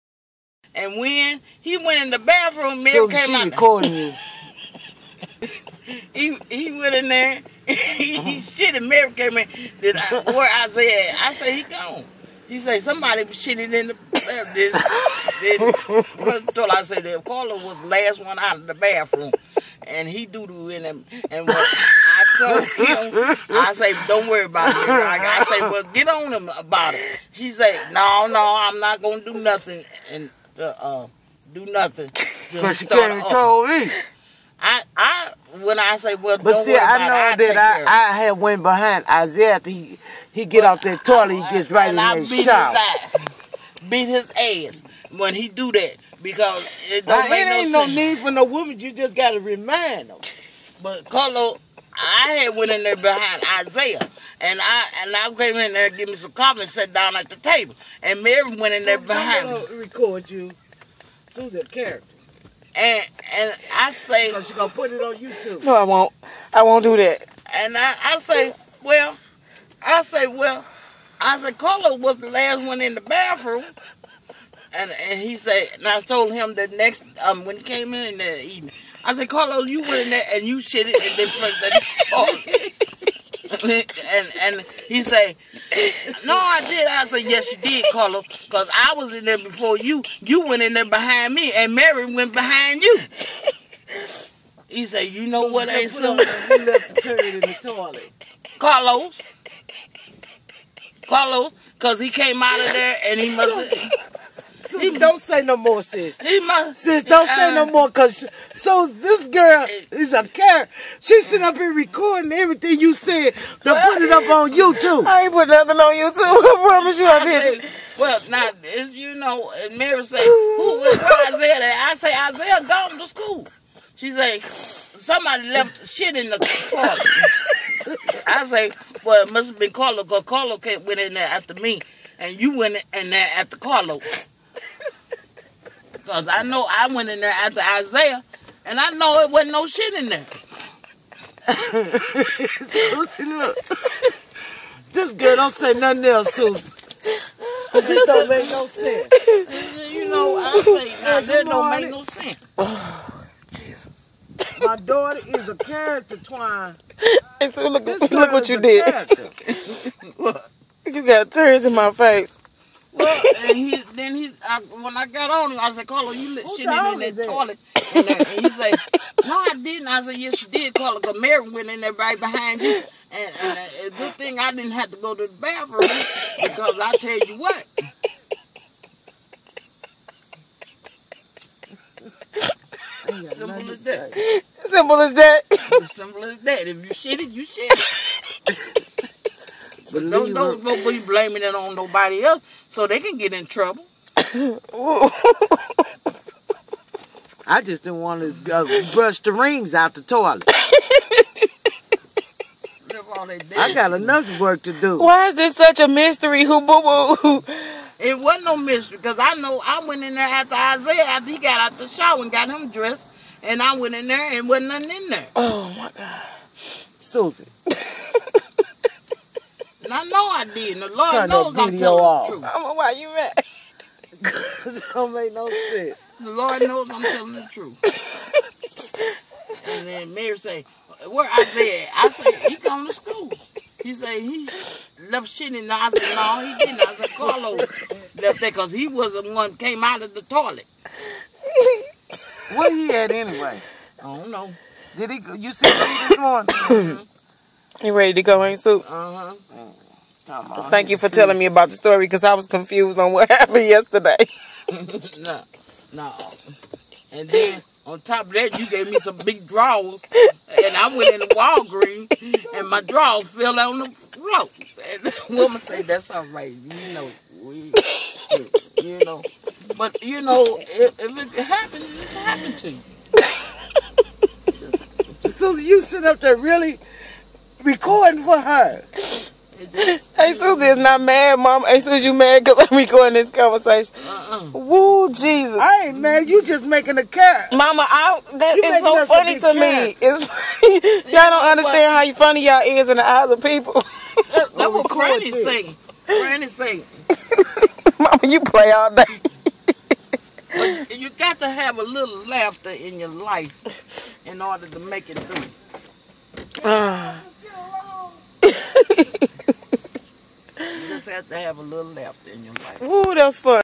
Who left boo boo in the toilet?
My aunt tells her side of the story about what was found in the toilet.